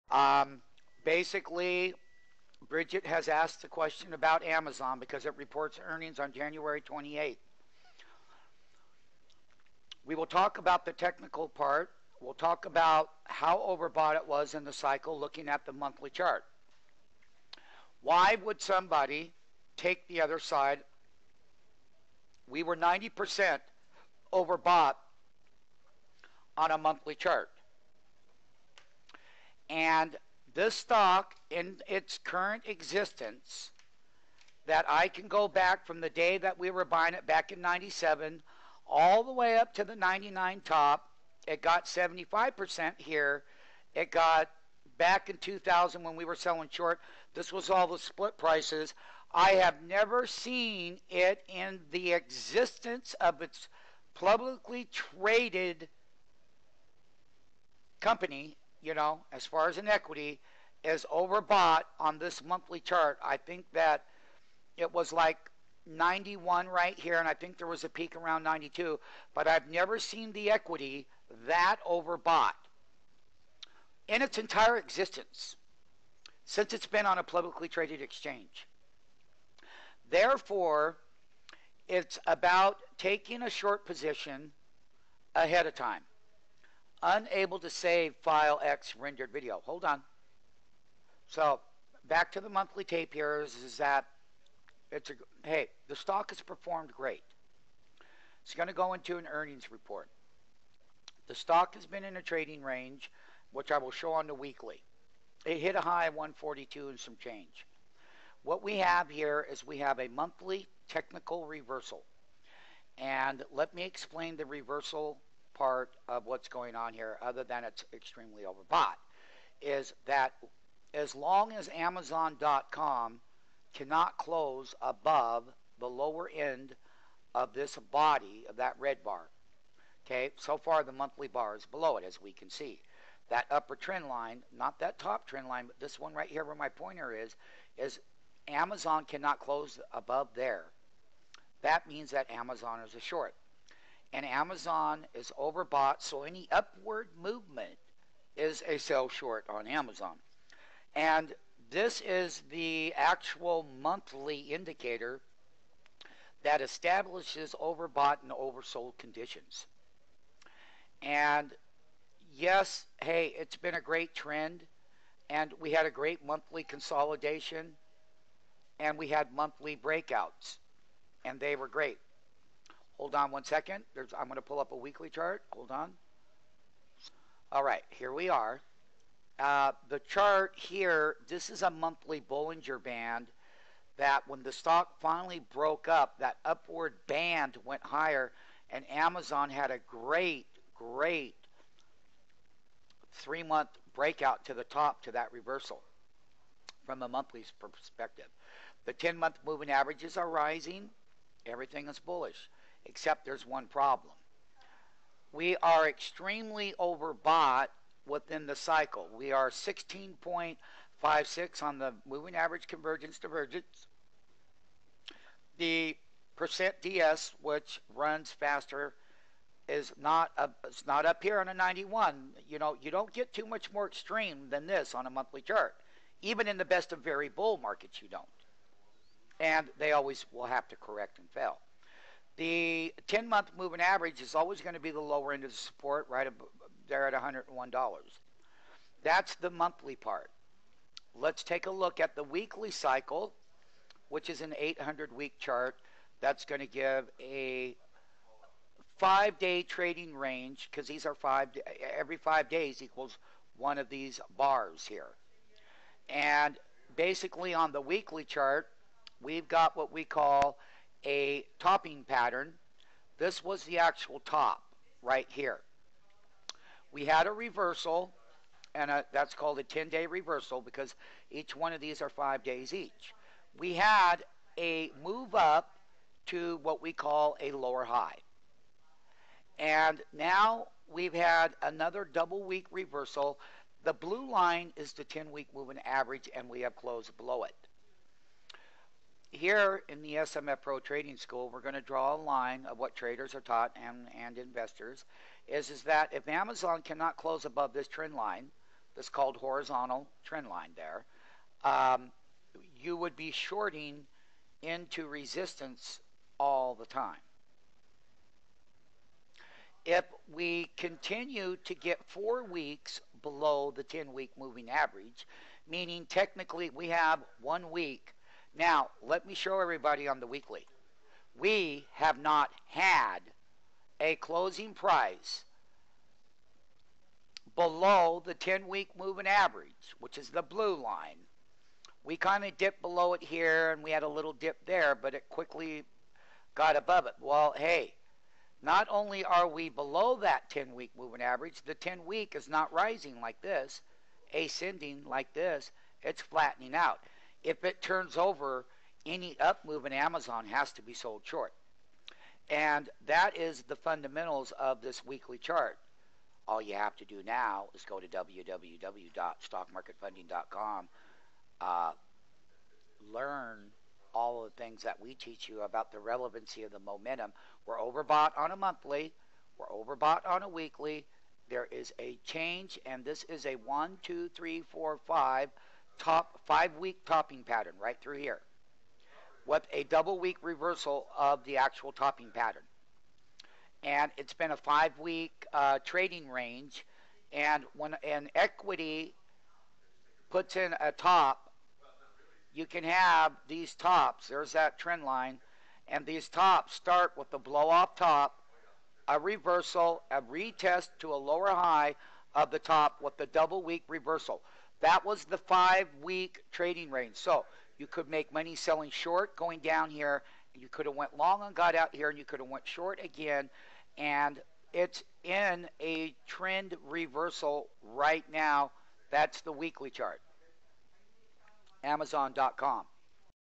Stock Market Trend Analysis Video Monthly Chart on AMZN
Stock Market Trend Analysis Video Monthly Chart on AMZN In this monthly stock chart example, StockMarketFunding.com highlights the current overbought stock market trend analysis in this stock video lesson of AMZN.